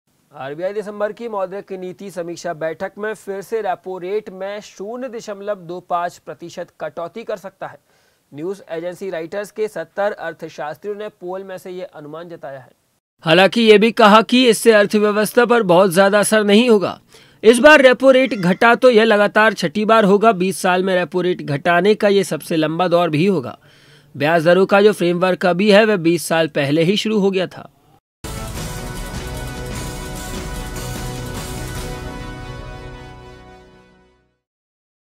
MUMBAI आरबीआई रेपो रेट घटा
आरबीआई 3-5 दिसंबर की मौद्रिक नीति समीक्षा बैठक में फिर से रेपो रेट में 0.25 प्रतिशत कटौती कर सकता है। न्यूज एजेंसी रॉयटर्स के 70 अर्थशास्त्रियों के पोल में ये अनुमान जताया गया है।